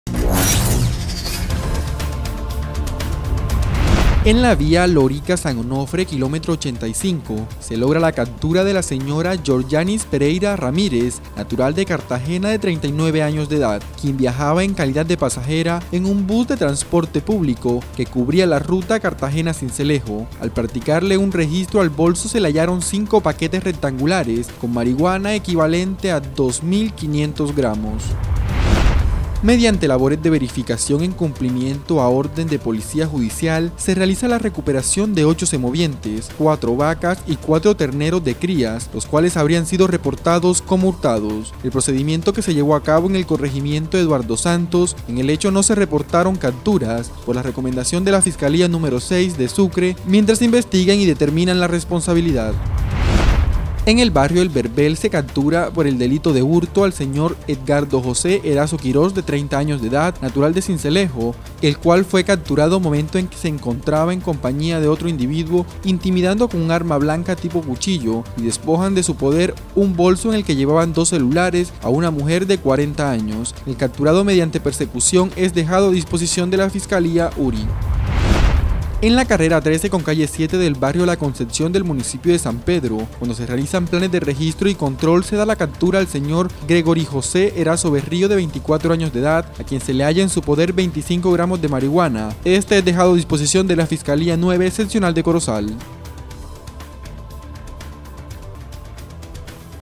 RESUMEN JUDICIAL 20/02/2020
La policía en sucre logró la captura de una mujer a la que le hallaron en su poder 5 paquetes de marihuana, esta y otras noticias en nuestro resumen judicial.